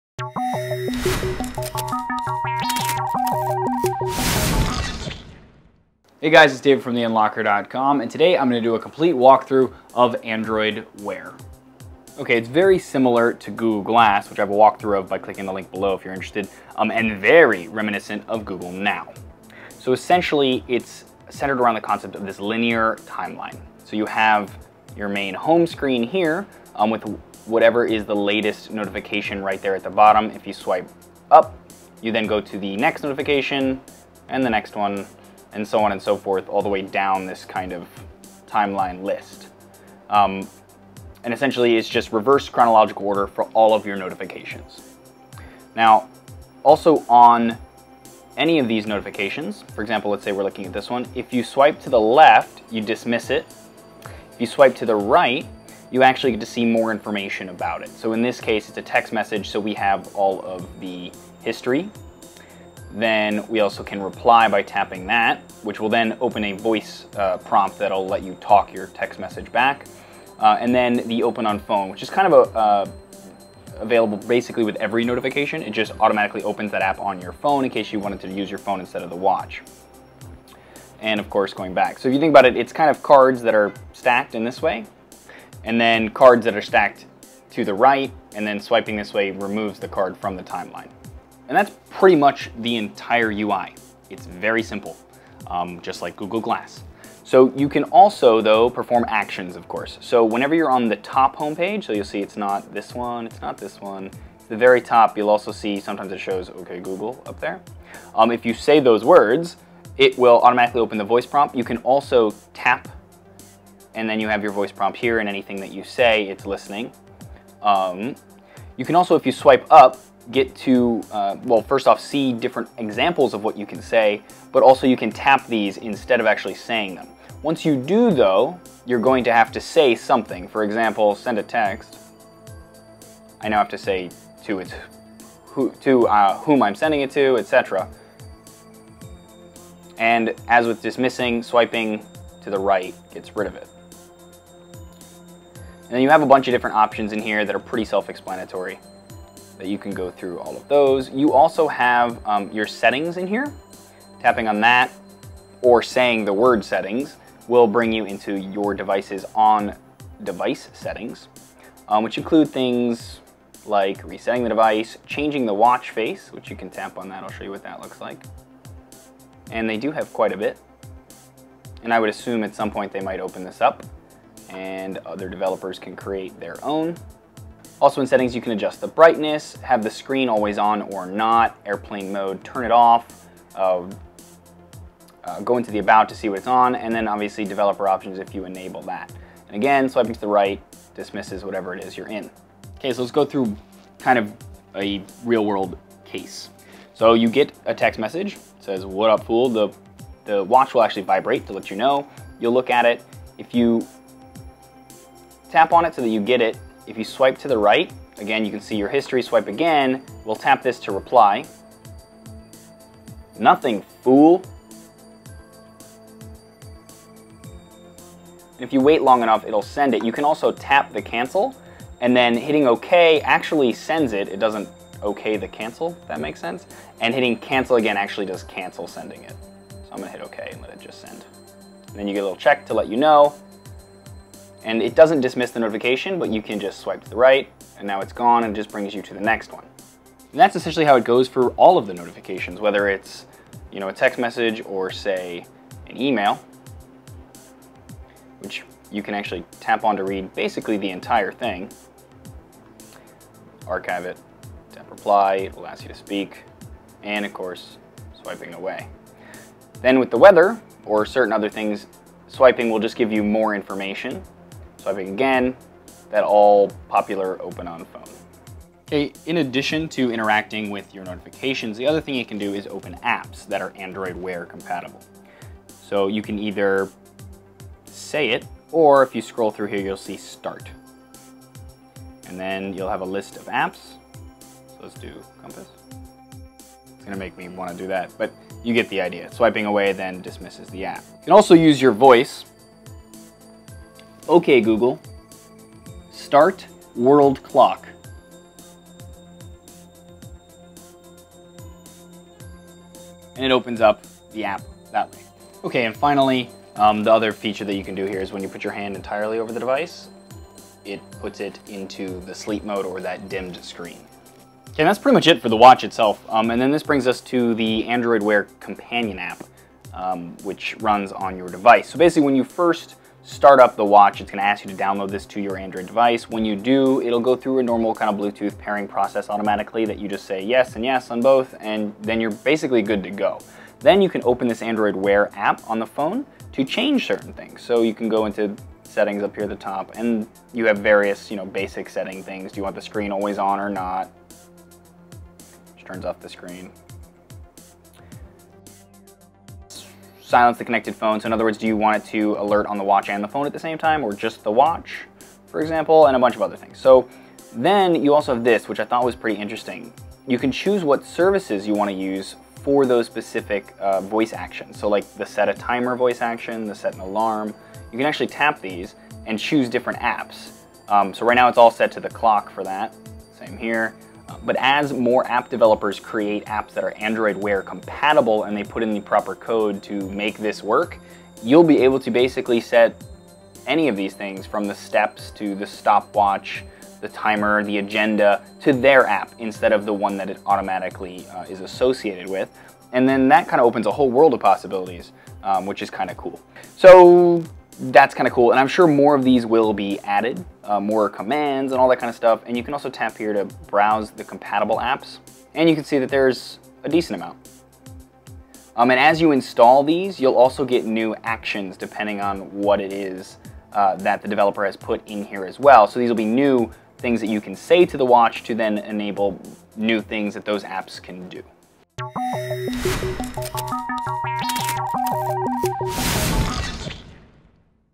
Android Wear Walkthrough
While I have the LG G Watch for a limited time, I figured it might be a good time to do a walkthrough of Android Wear for anyone still wondering, "what the heck does it do?". Without further ado, here's what the heck it does...